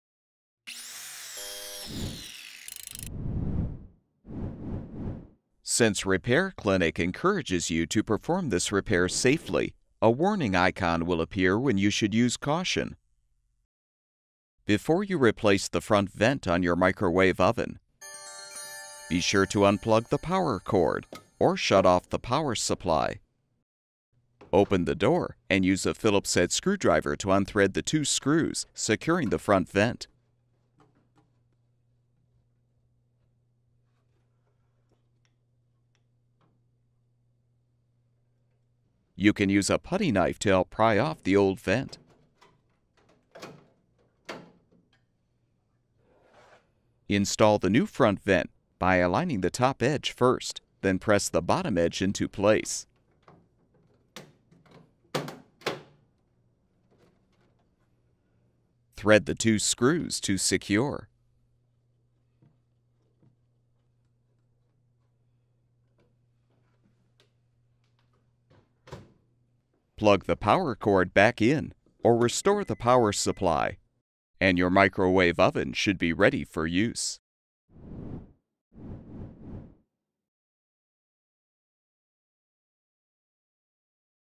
Frigidaire Microwave Oven Front Vent Replacement 5304509490
This video provides step-by-step repair instructions for replacing the front vent on a Frigidaire microwave oven (Model FFMV1645TSA). The most common reason for replacing this part is if it is damaged or missing.

Frigidaire